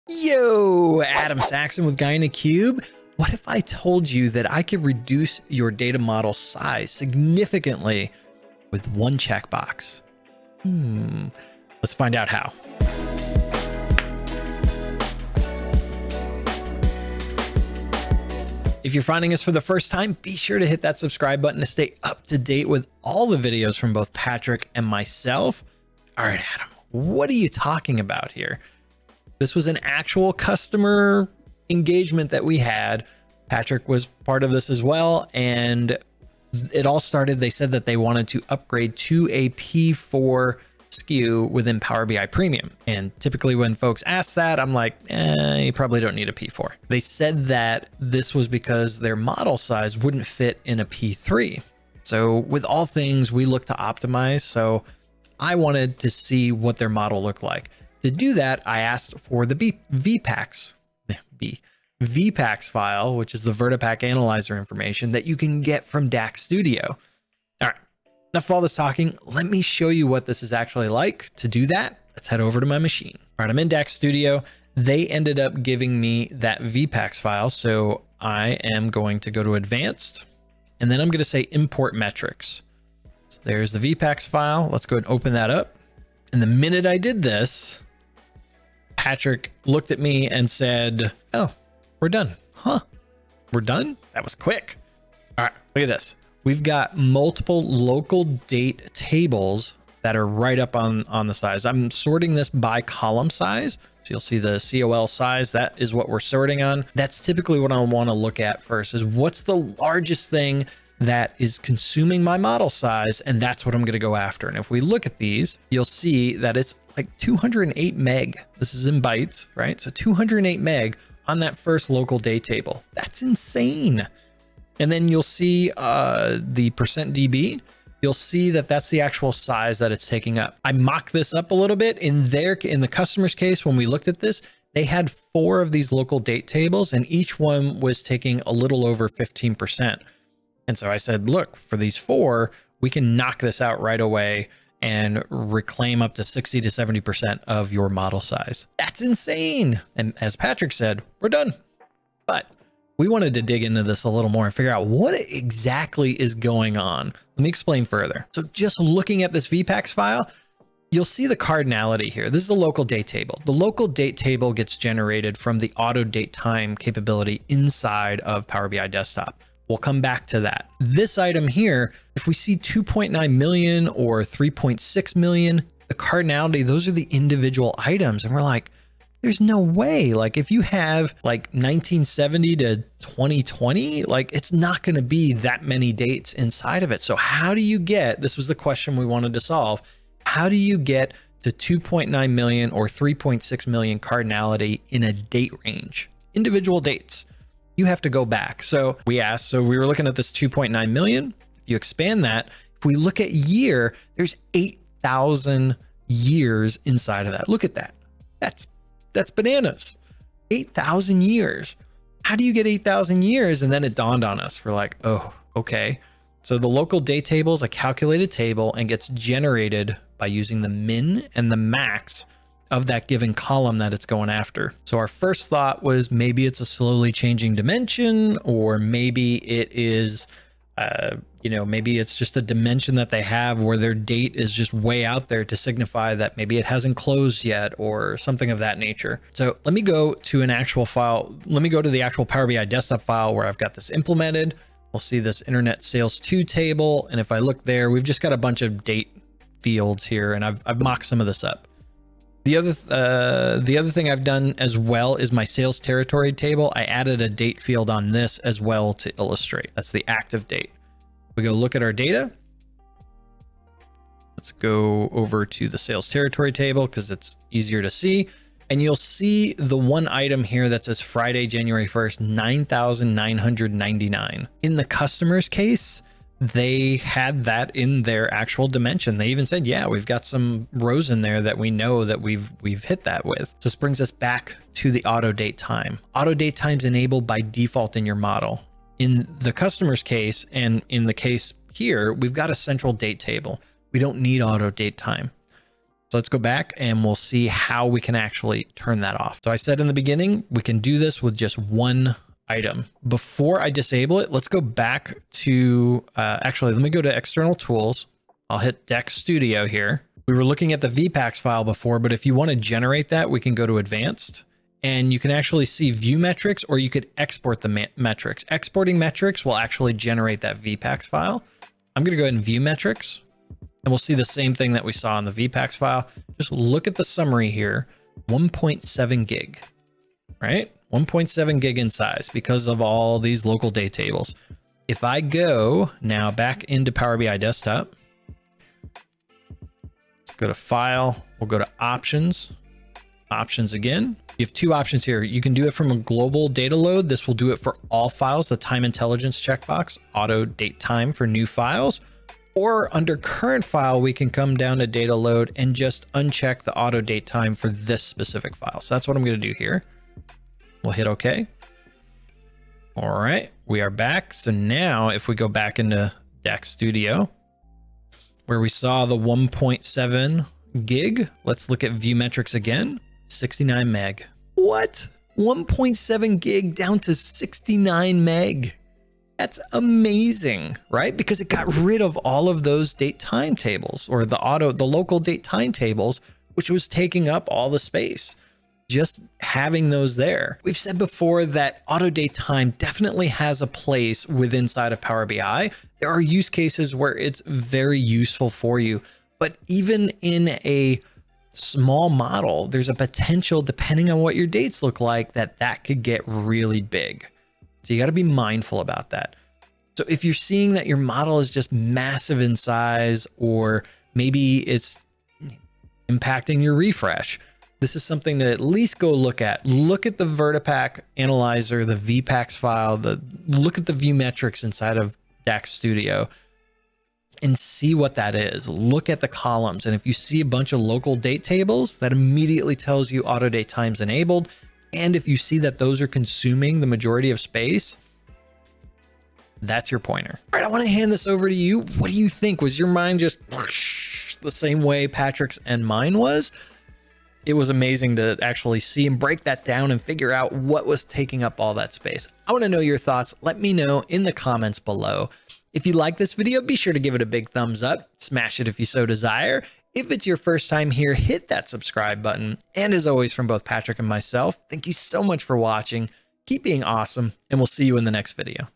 REDUCE Power BI dataset size by 60% with ONE CHECKBOX???
What if I told you that you could potentially reduce your Power BI dataset size by up to 60-70% with ONE CHECKBOX??? This was crazy and we show you what is going on! (results will vary)